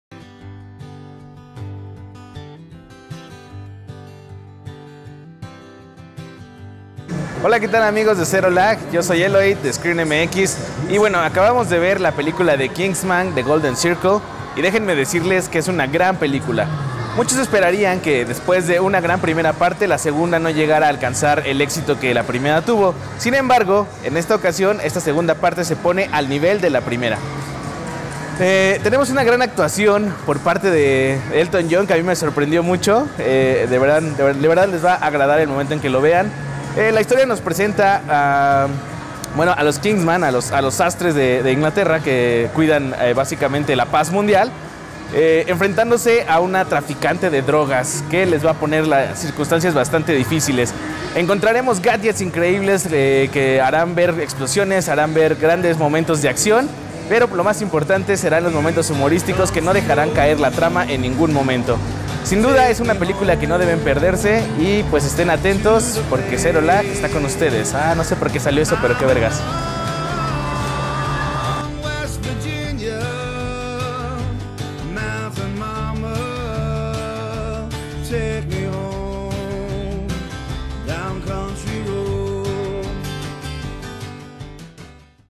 OPINIÓN - KINGSMAN: El Círculo Dorado | Kingsman 2
Nuestro amigo Ulises Valdez (@Eloith) nos da su opinión de Kingsman: El Círculo Dorado. ¿Qué le habrá parecido? ¡Entérate en nuestro video!